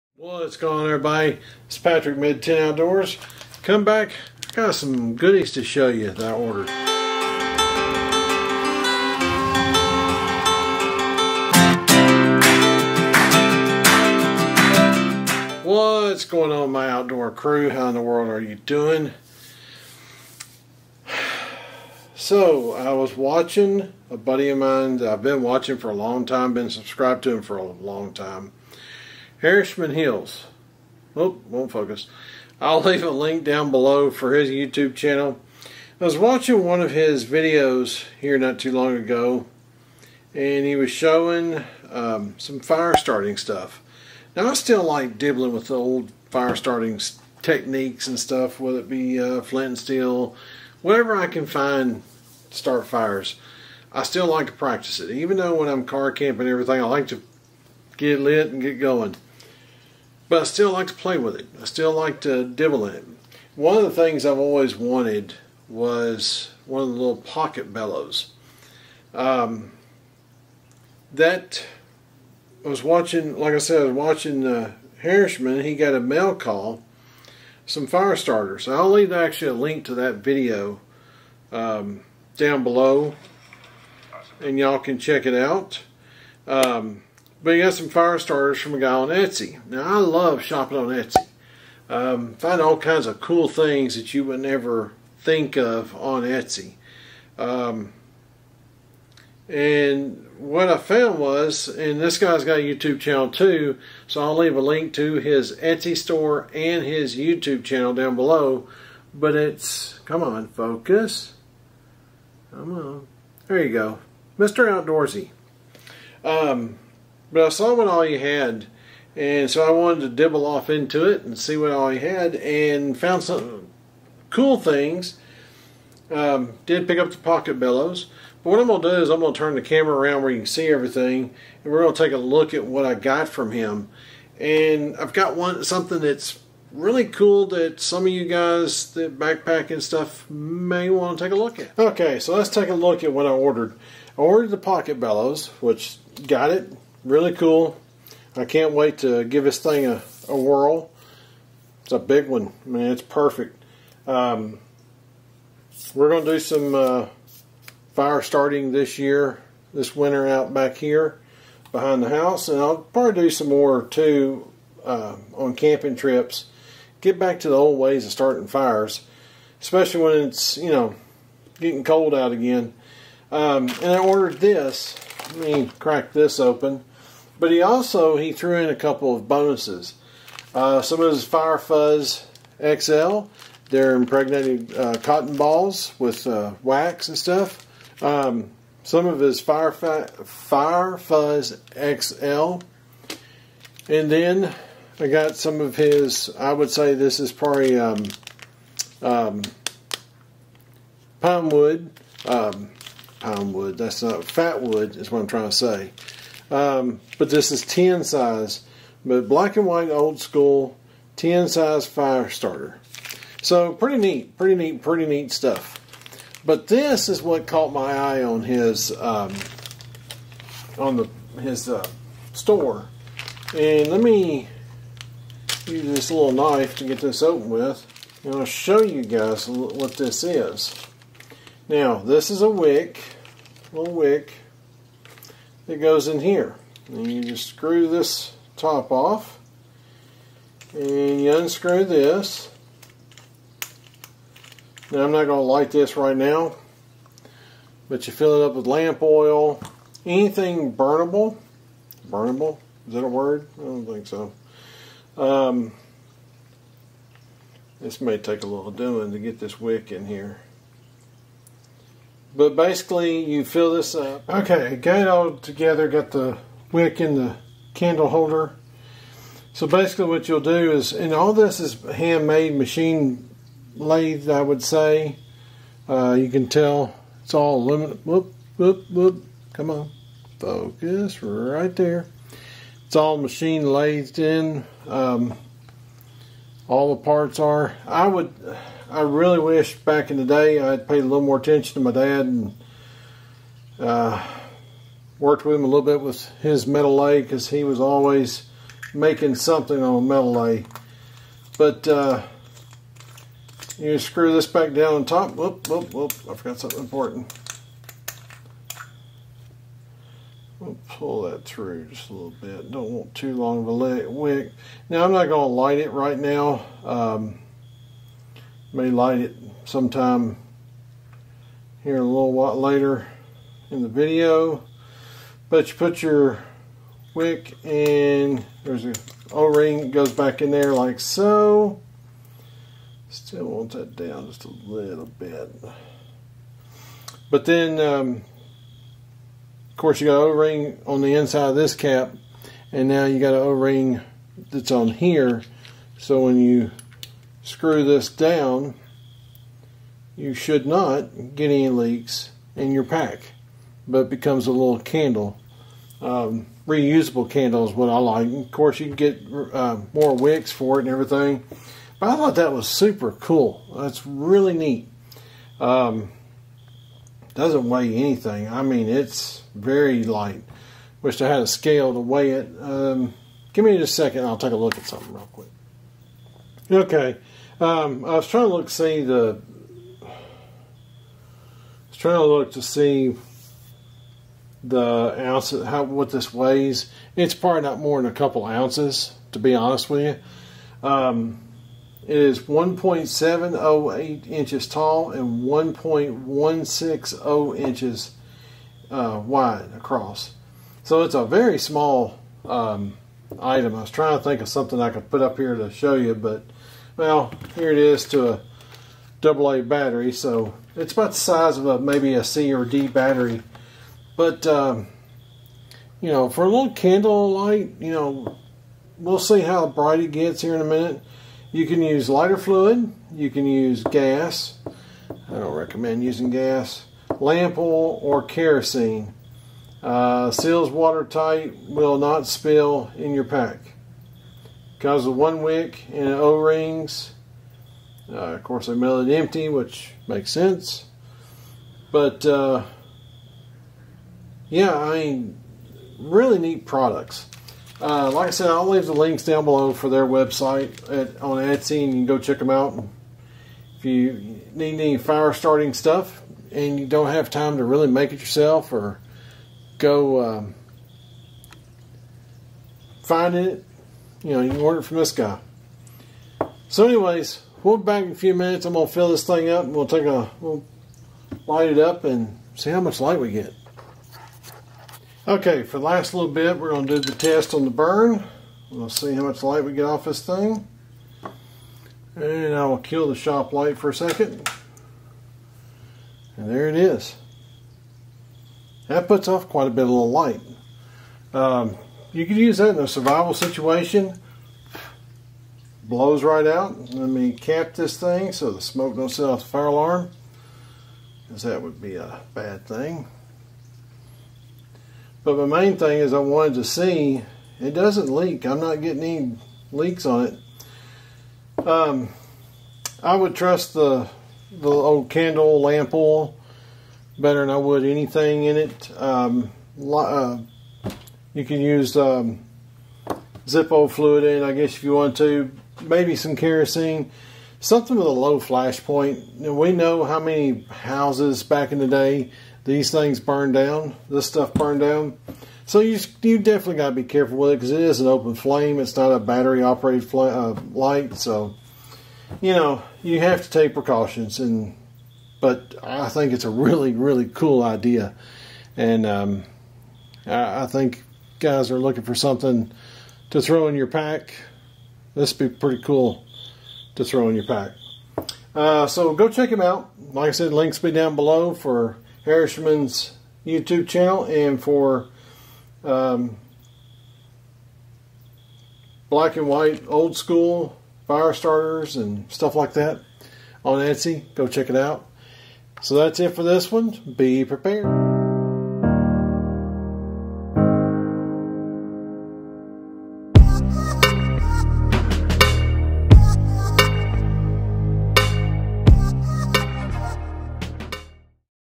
Black and White Fire Starters
In this video, we take a look at Black and White Fire Starters and their Fire Mini Lamp.

Amazon Link
Nomadic Gear Backpack Clip

Other Cameras used
Canon SL3
Gopro Hero Black 5 & Sessions 4